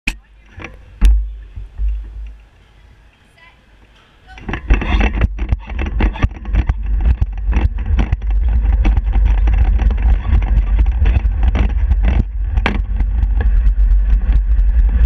Ben Brown Indoor 40m 5 20/12/15
10th sprint of the day pushed 7.98 seconds hit 24.1km/hr